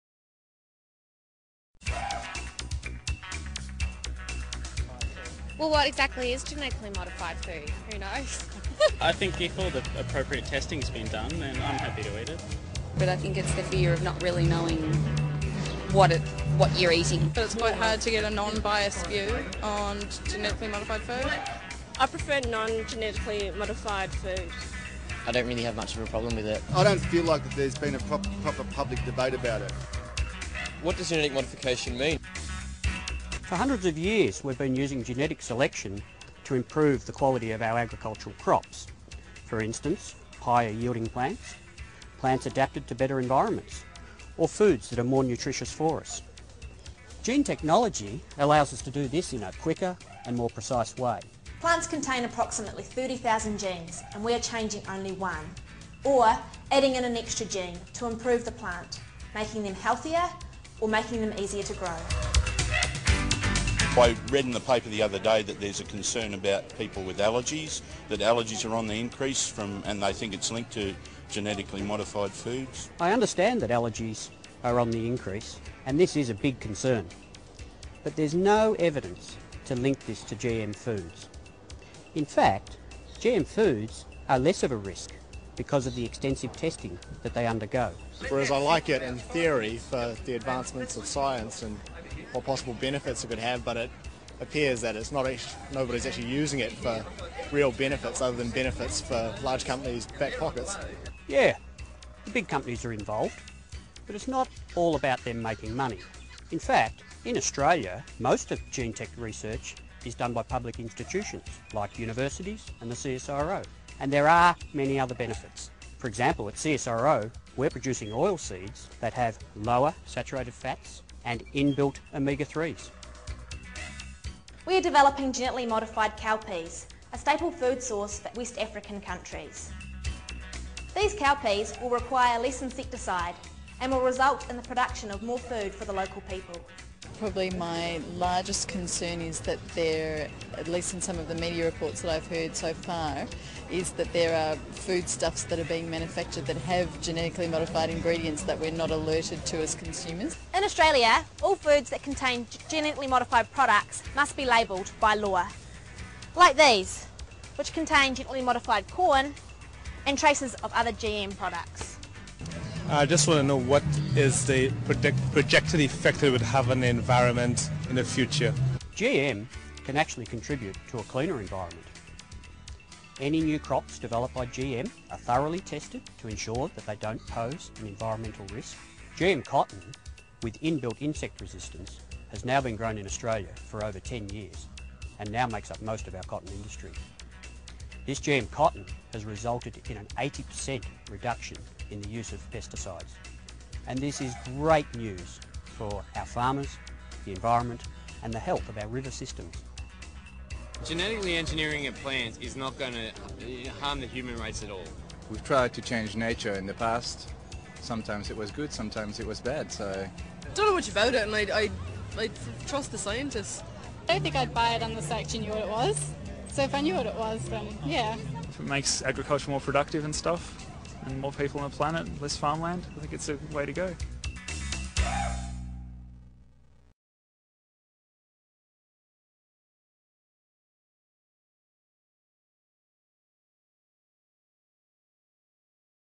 Australian GM foods and crops
The public comments on genetically modified foods and crops in Australia.